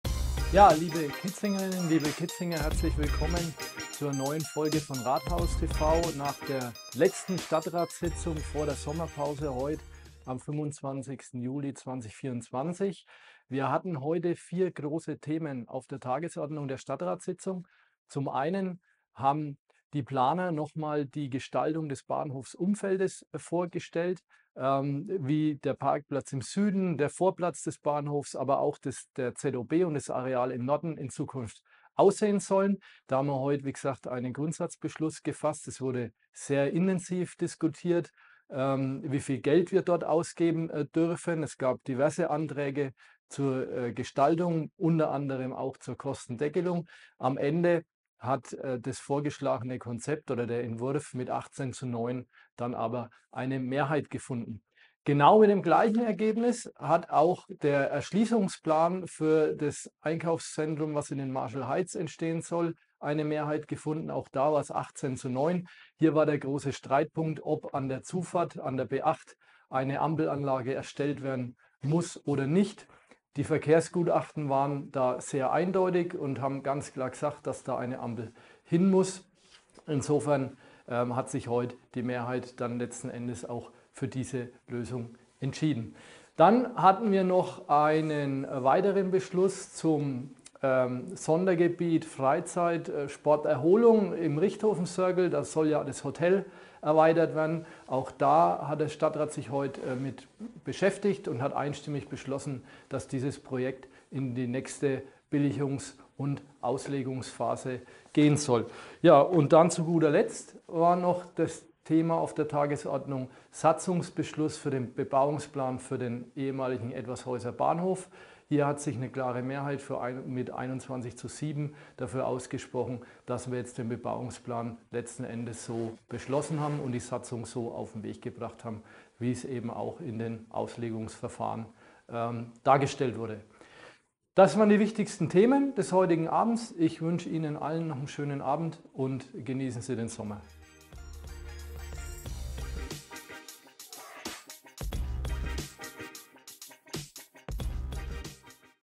Kitzinger Rathaus TV | Stadtratssitzung vom 25. Juli 2024
Oberbürgermeister Stefan Güntner informiert über die wichtigsten Themen der Stadtratssitzung vom 25. Juli 2024. Auf der Tagesordnung standen folgende wichtige Themen:
00:00 - 00:17 Begrüßung
00:17 - 00:54 Neugestaltung Bahnhofsumfeld
00:54 - 01:26 Einkaufzentrum Marshall Heights
01:26 - 01:48 Erweiterung Hotel im Richthofencircle
01:48 - 02:29 Neue Gartenstadt Etwashausen